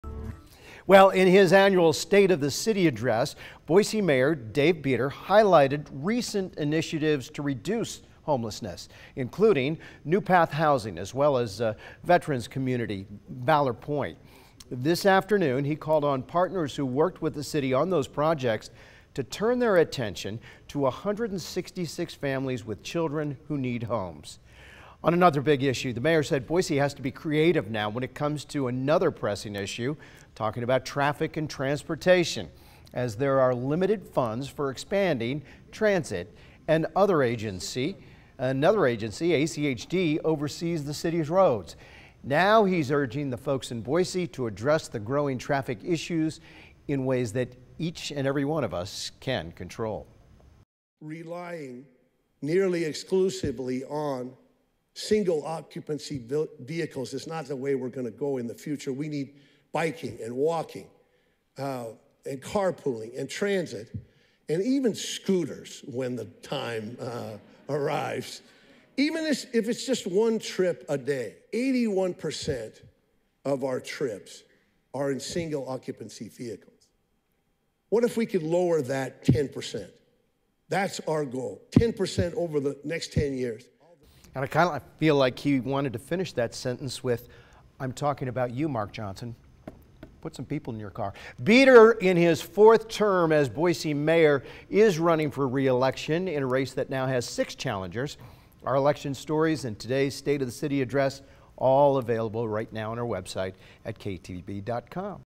Bieter delivers State of the City address
Bieter says residents can do more to ease the traffic problems in the city.